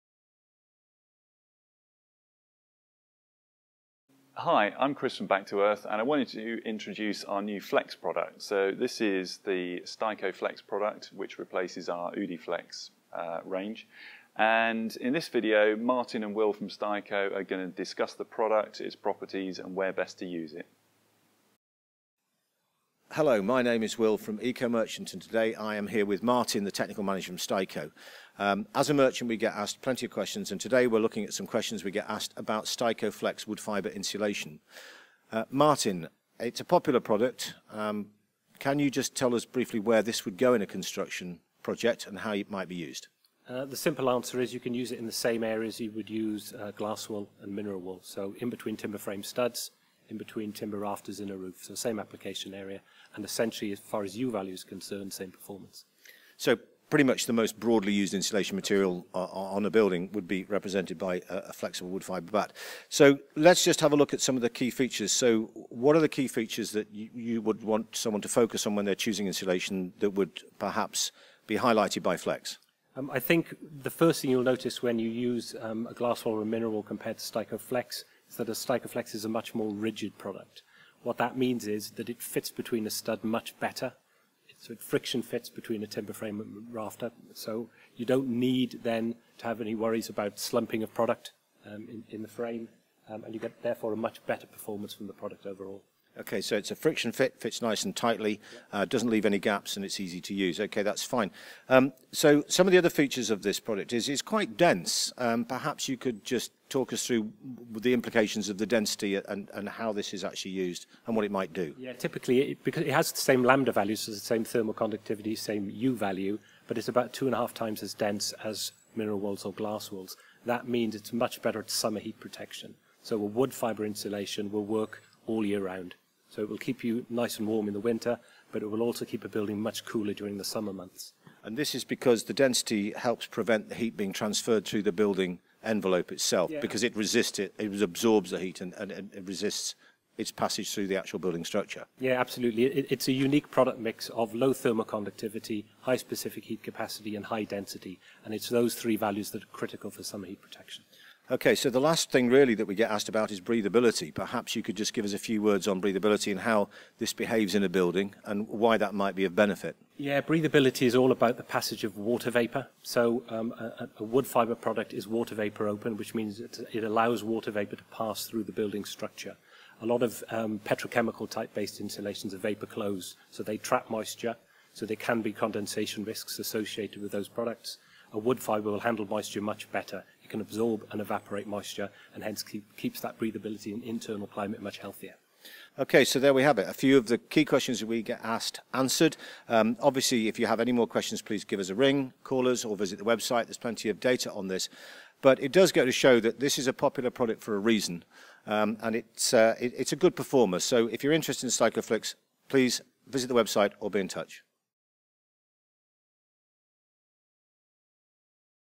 SteicoFlex flexible wood fibre insulation
This video is about our new flexible wood fibre insulation producty, SteicoFlex. Will Kirkman & Martin Twamley talk about Steicoflex, it's properties and how to use it.